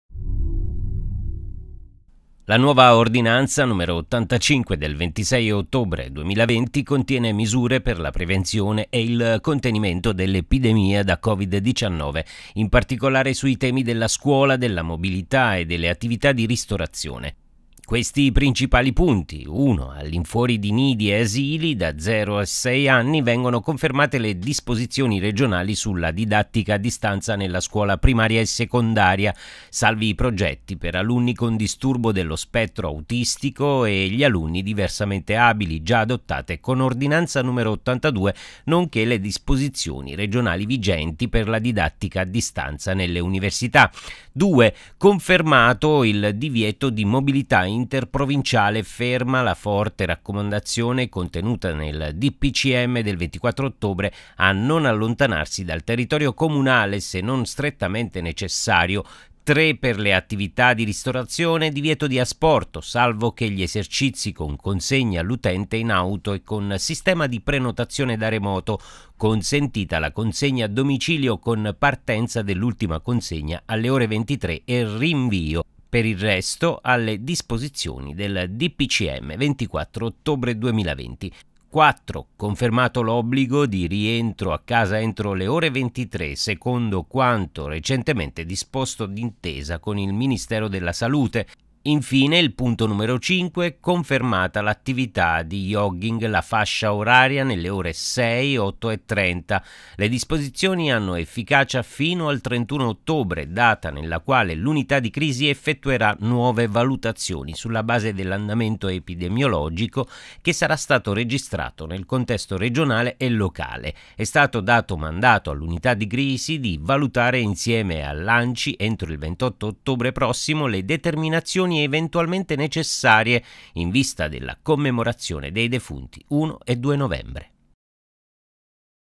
Campania: Nuova ordinanza Scuola, mobilità e ristorazione.
La nuova ordinanza n. 85 del 26/10/2020 contiene misure per la prevenzione e il contenimento dell’epidemia da COVID-19, in particolare sui temi della scuola, della mobilità e delle attività di ristorazione.